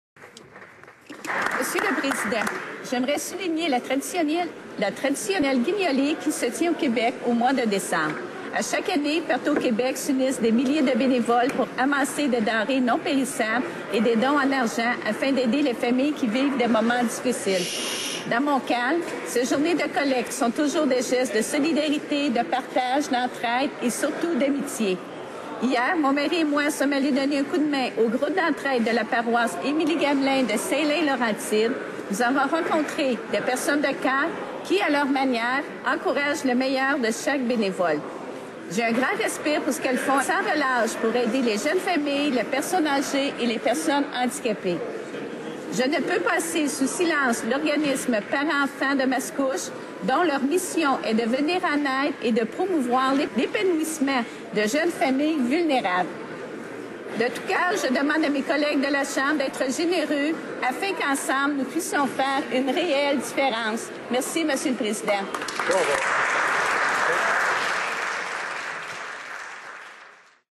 Manon Perreault souligne la guignolée annuelle de Lanaudière.
Déclaration dans la chambre des communes, le 2 décembre 2013.